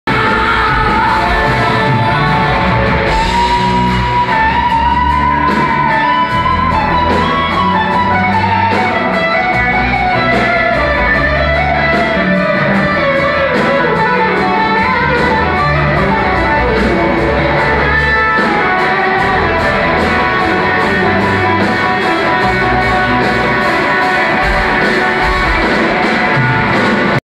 Jonny Lang Guitarsolo 06.Oct 2013 Zurich X-Tra
Jonny Lang in Concert at X-Tra Zurich Switzerland  06. Oct. 2013